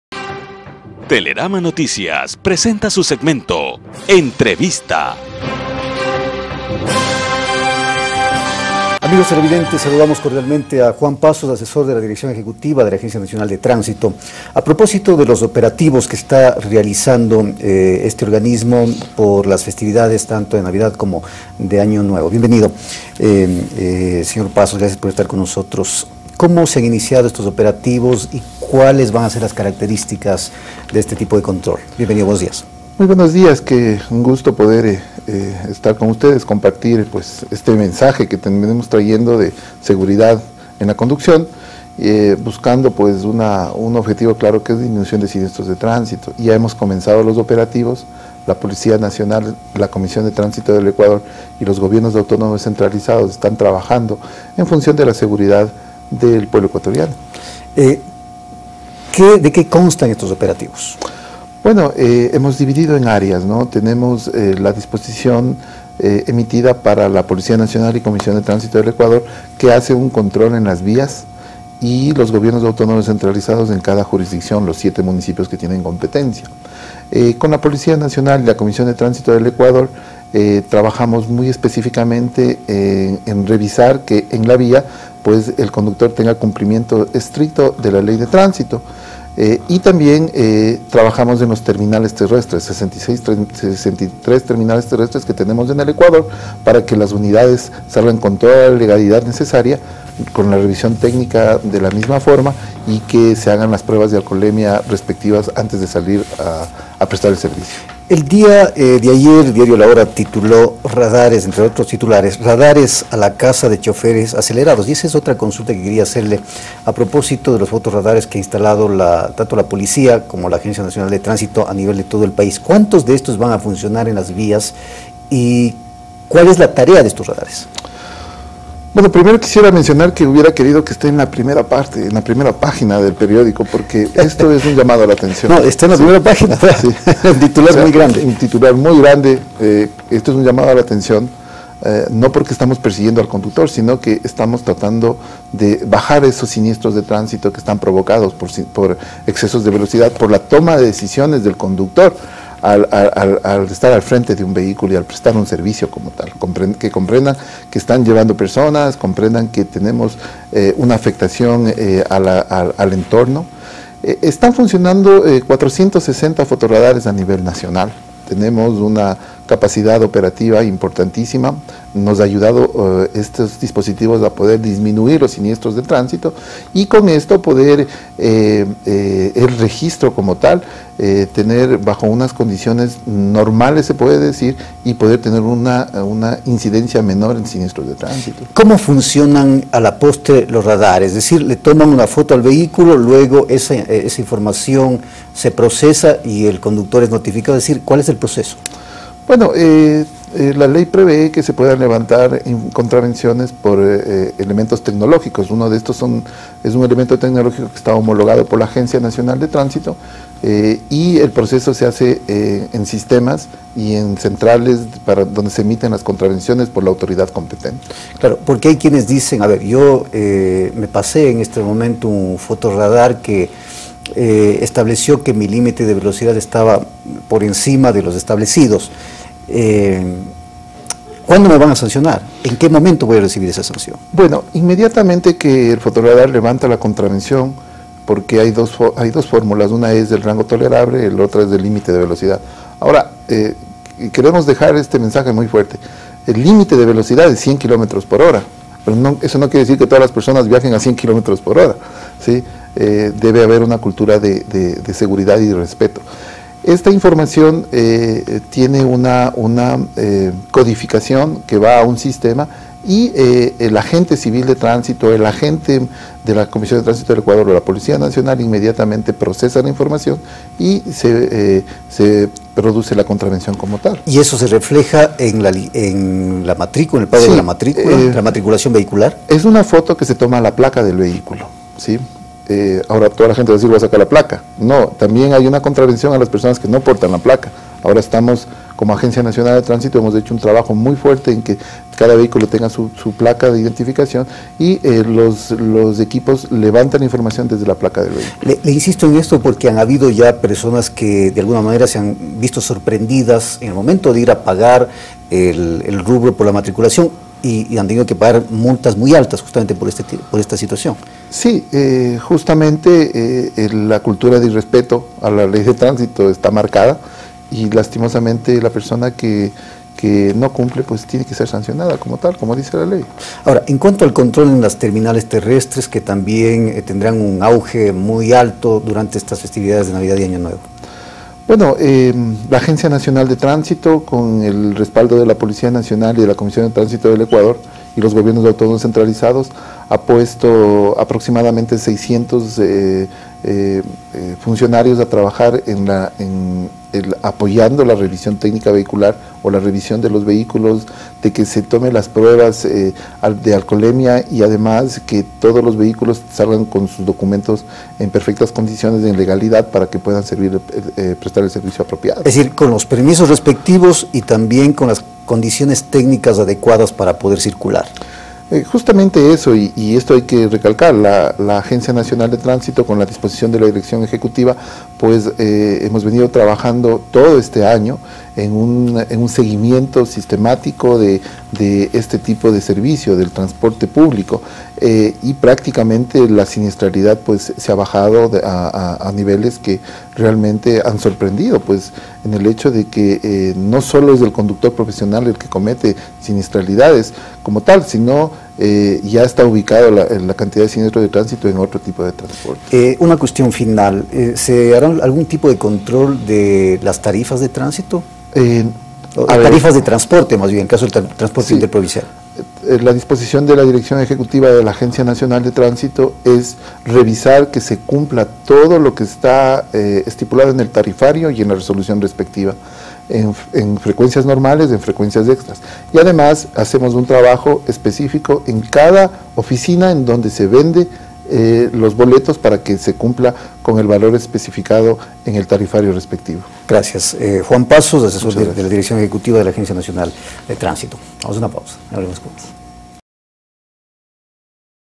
Juan Pasos
Asesor dirección ejecutiva ANT
Tema:  Operativos de tránsito por navidad y fin de año